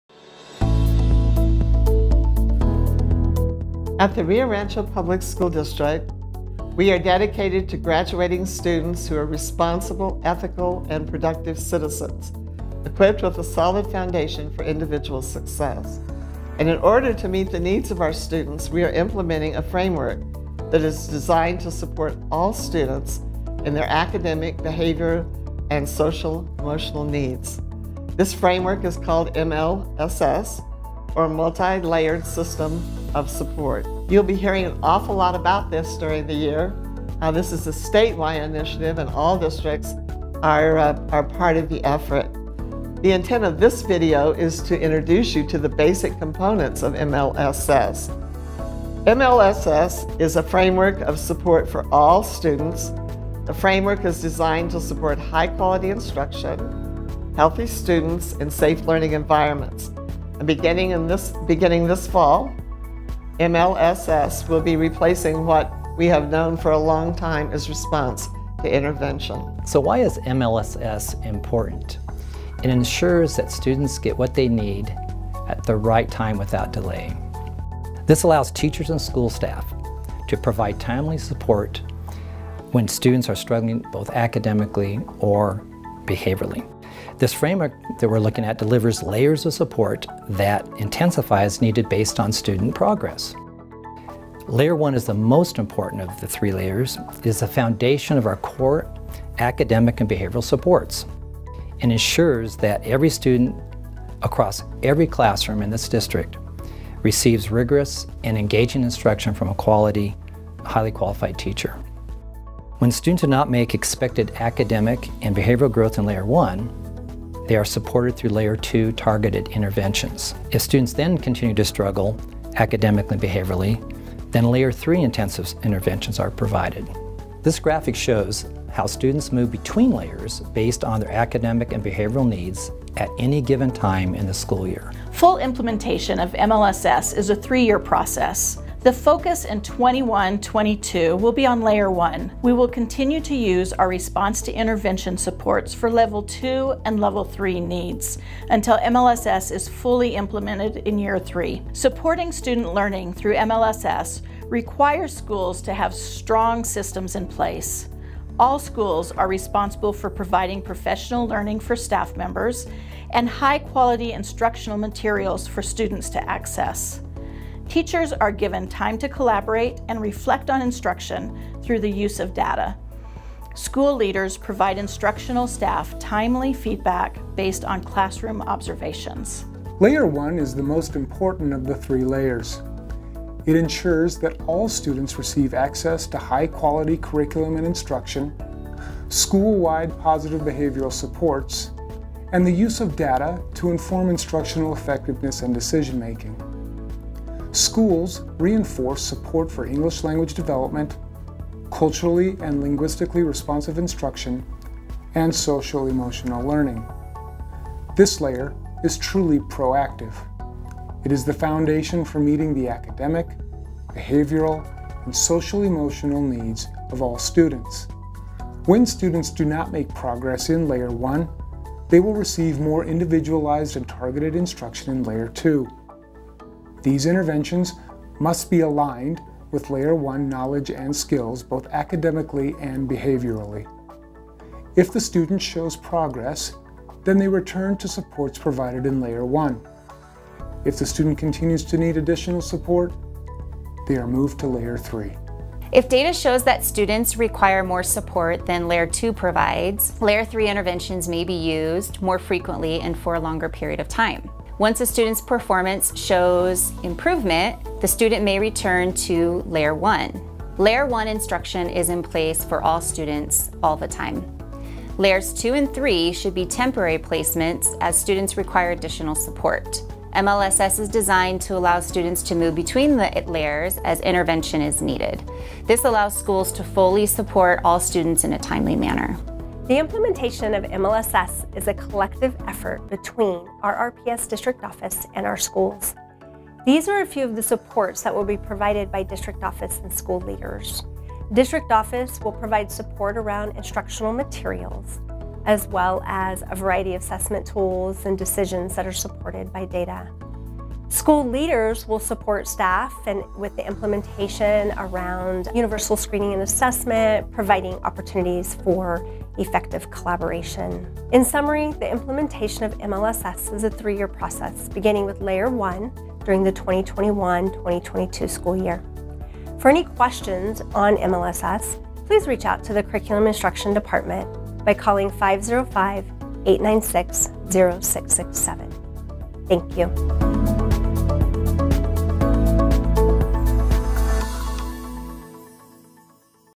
Multi-Layered System of Supports (MLSS)
Multi-Layered System of Supports, or MLSS, is a framework of support for all students. This framework is designed to support high-quality instruction, healthy students, and safe learning environments.

Beginning in fall of 2021, MLSS will be replacing what has been referred to as Response to Intervention. This video is intended to teach you more about MLSS.